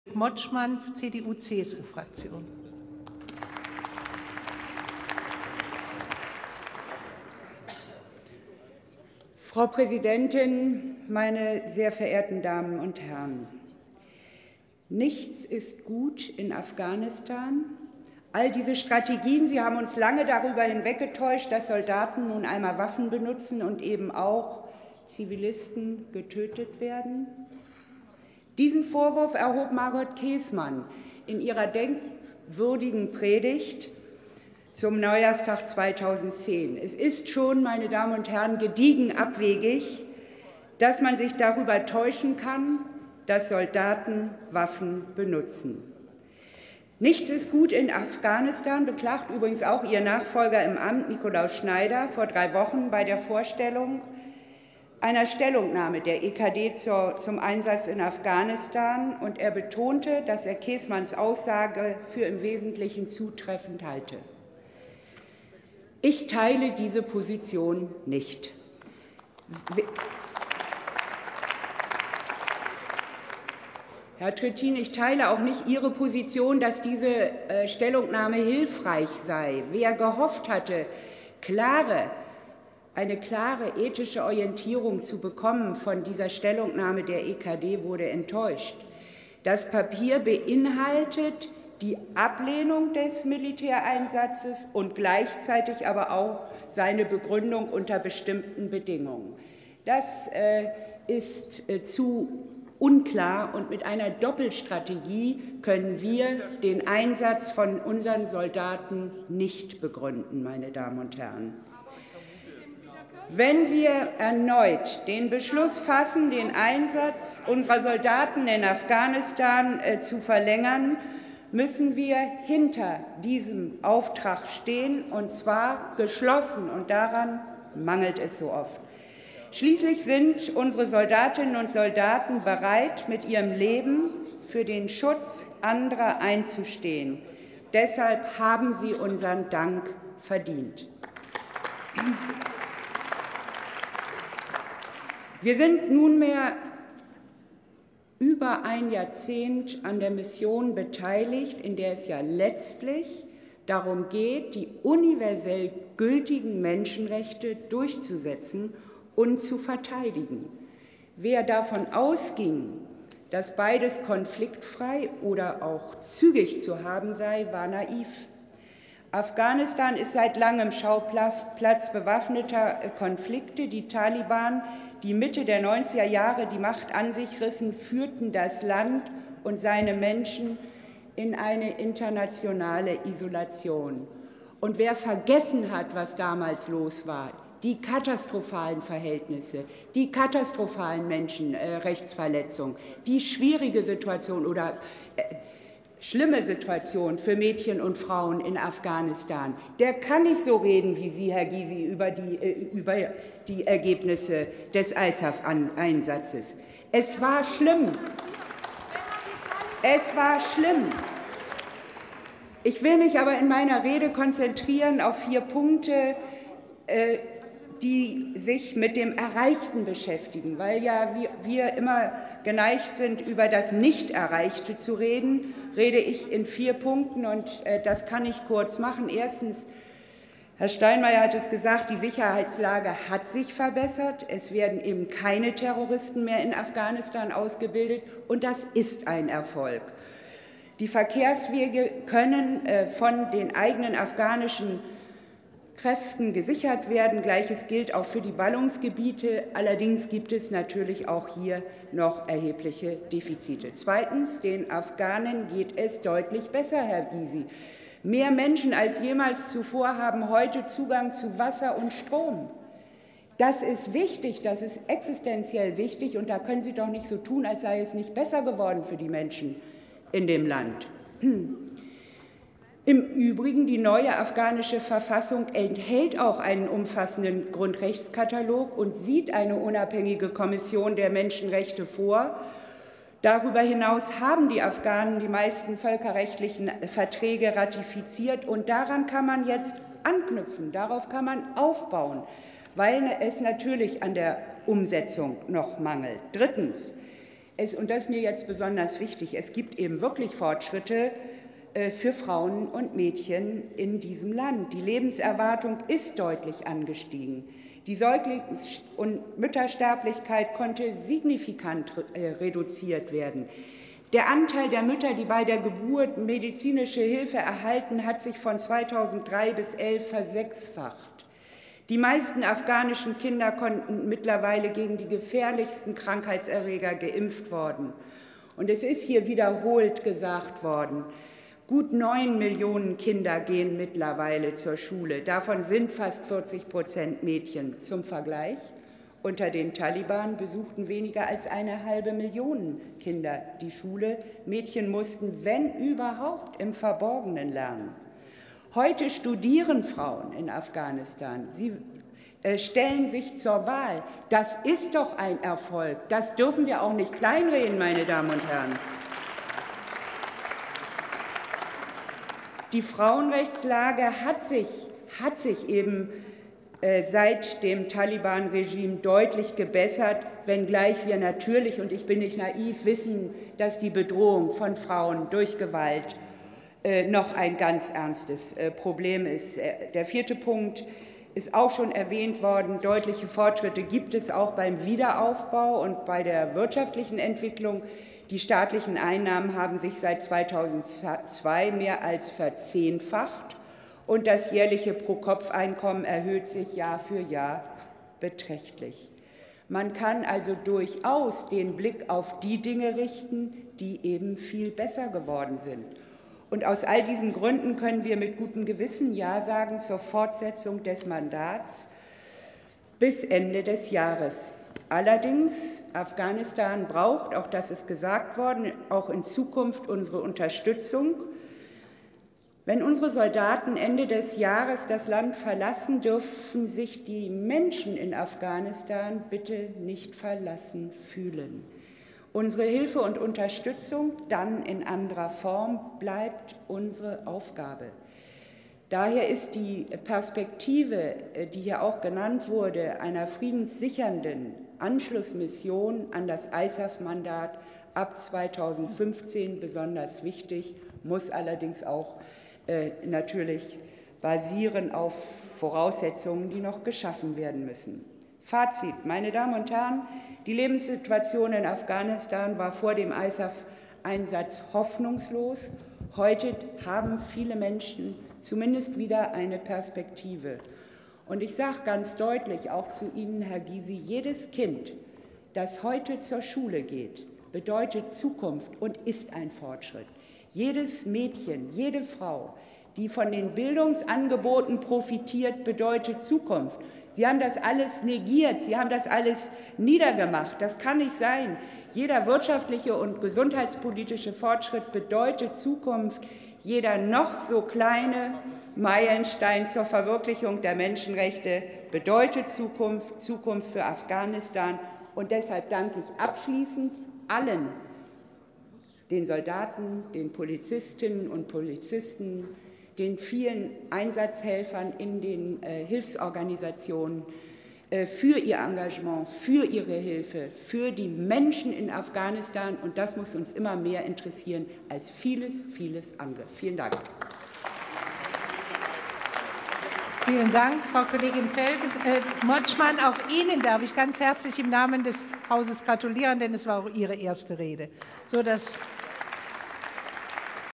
Rede von Elisabeth Motschmann im Bundestag zum ISAF Einsatz in Afghanistan
Sehen Sie hier die erste Rede von Elisabeth Motschmann im Deutschen Bundestag. Als ordentliches Mitglied im Auswärtigen Ausschuss redet Sie über die Fortsetzung der Beteiligung deutscher Soldaten an dem Einsatz der Internationalen Sicherheitsunterstützungstruppe (ISAF) in Afghanistan.